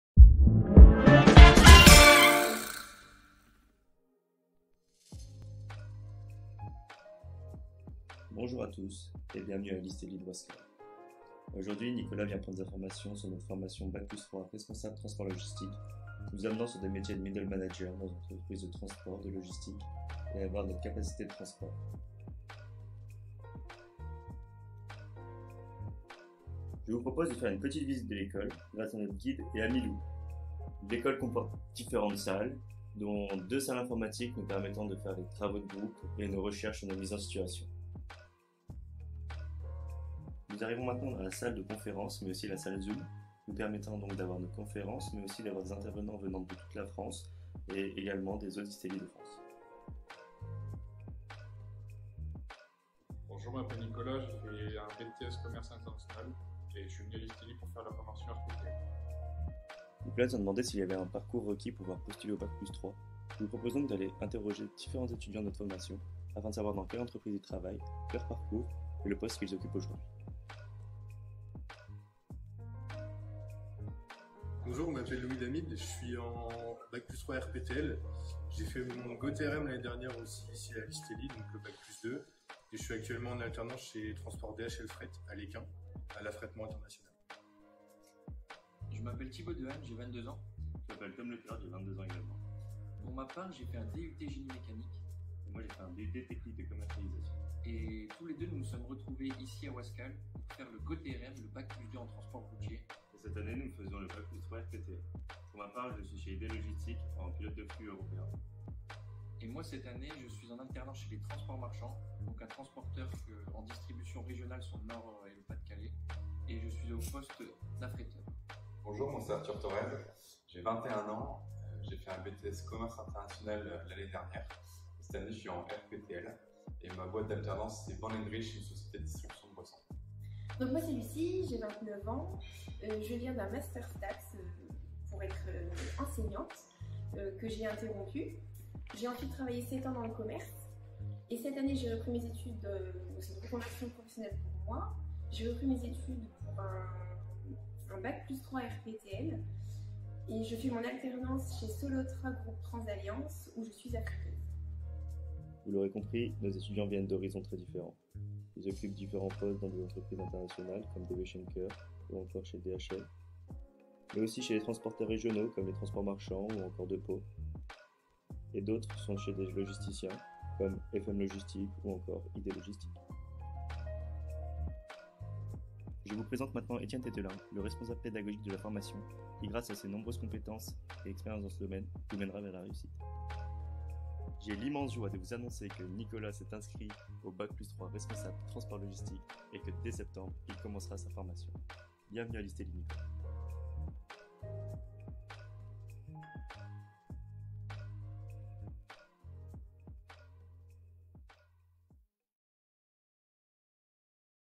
Je filme ma formation - RPTL - Responsable Transport & Logistique
Félicitations aux étudiants RPTL - RESPONSABLE TRANSPORT & LOGISTIQUE dont la vidéo a été sélectionnée au concours national

Découvrez leur formation et encouragez les en votant pour leur vidéo !